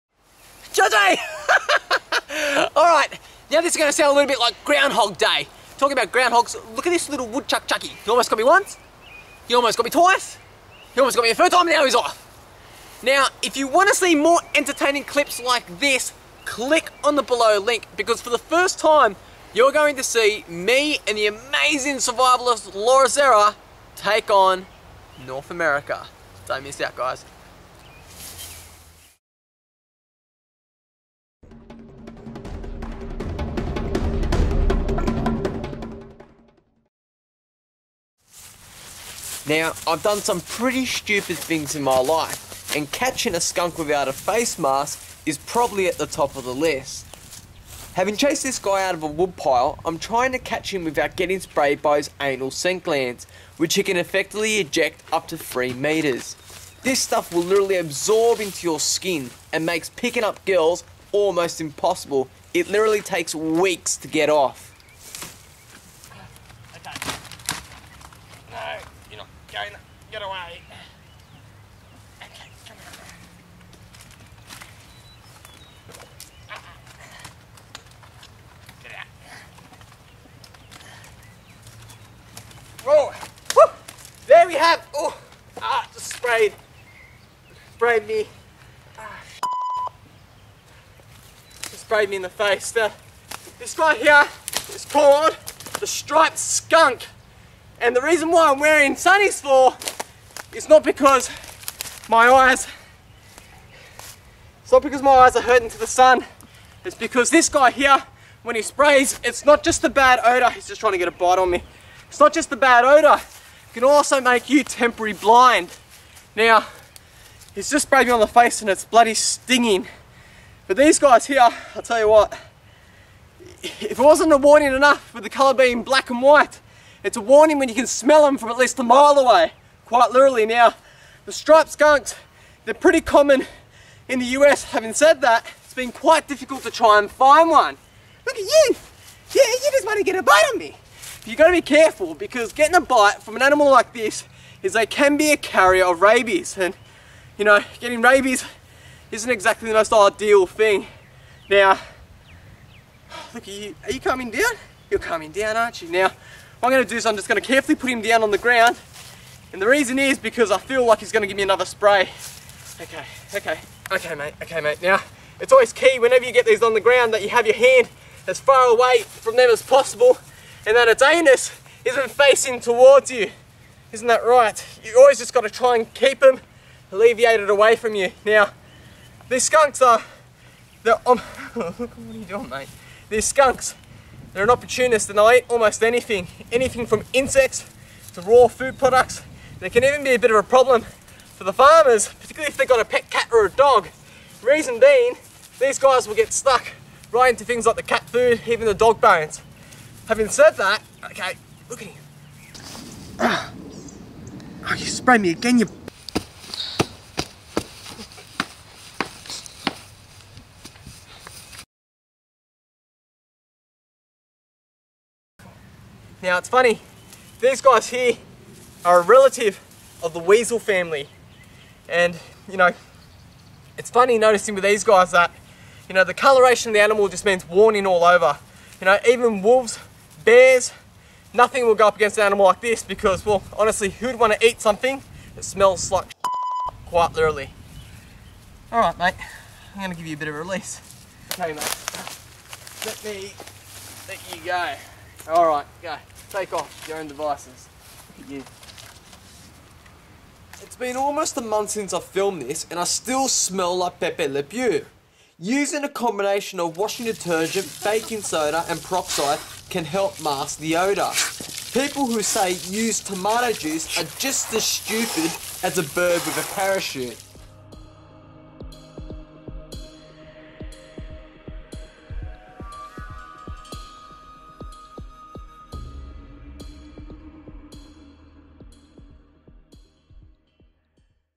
DON'T ever catch a SKUNK - with Andrew Ucles
The common striped skunk! This was my first attempt at both capturing and handling this black and white animal. I'll put to you this way, in a bear's life how many skunk's do you think he would consume in his life time?... the answer is simple!

Having been sprayed several times by my little black and white friend, it wasn't long until I noticed that the scent wasn't just a short term effect, rather it was a chemical which had absorbed into my skin and was now very much apart of my scent.

Over a week later I still smelled like a Skunk bomb, everything I touched, wore and ate now started to smell like skunk, and seriously the scent was now starting to psychologically effect me, try walking into a store smelling like Pepe Le Pew... not good.

The answer to the question how many skunks does a bear eat in his lifetime... the answer is... one.

Survival is all about learning from your mistakes.

[The following footage was complied by an experienced individual(s) - no attempt should be made to reenact or recreate the following footage. No animals within the following footage were harmed. The footage depicts animals being restrained and no intent has been made to injure, harm or stress the animal(s). All animals used within the following footage were released back into their natural state. Viewers should not attempt the following outtakes without the appropriate permits and review of the relevant legislation and industry professional assistance.]